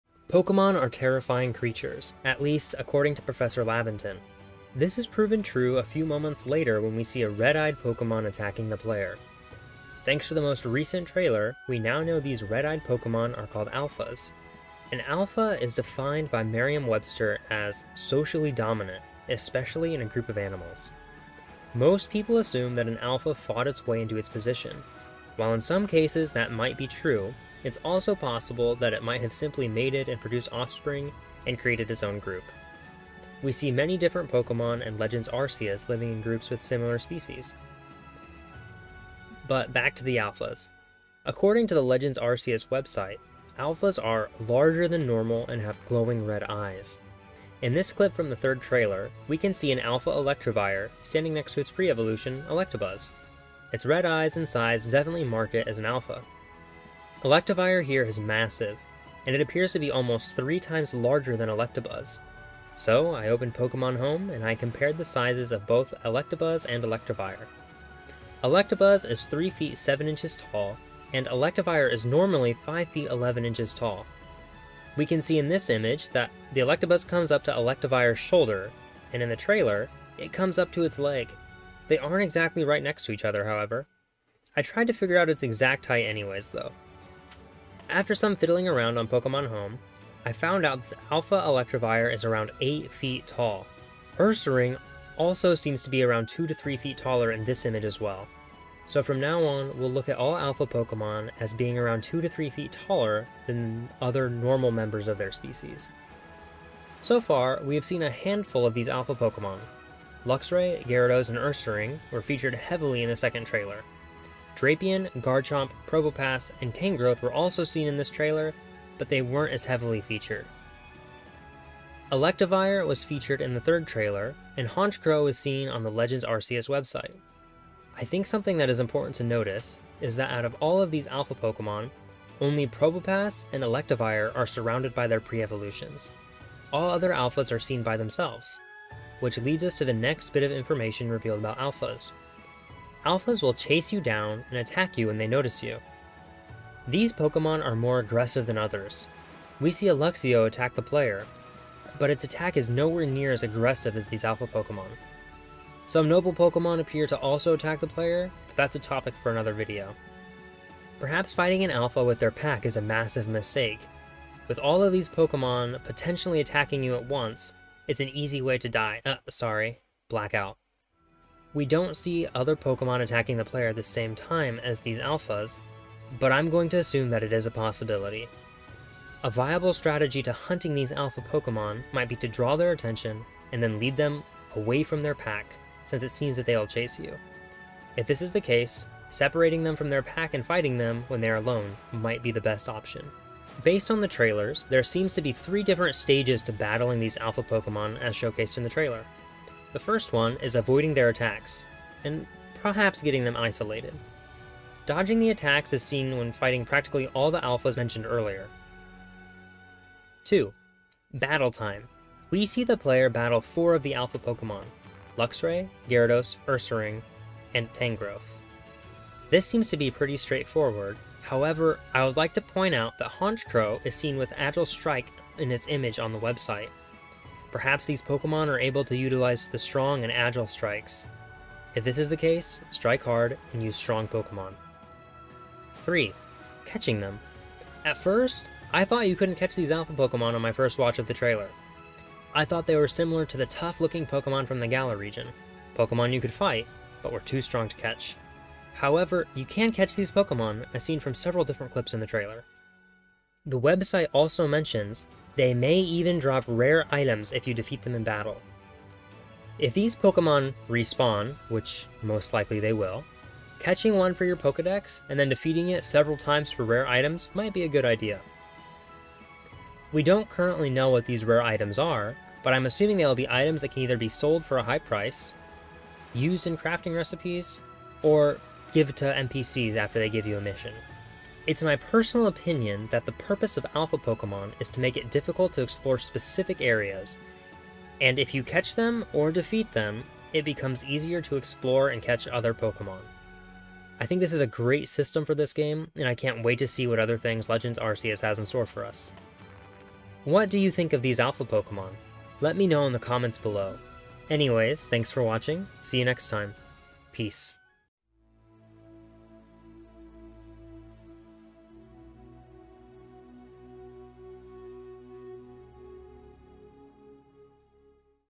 Alpha's Explained - Pokemon Legends Arceus
Legends Arceus still has many mysteries to unravel,  in this video I explain what little scraps of information about the Alpha Pokemon that will be present in the game. Most of this video is speculation, I am not saying these assumptions are 100% correct.

Also I tried a new style of Thumbnail for this video and I think it turned out really well. I might continue attempting to make thumbnails like this in the future.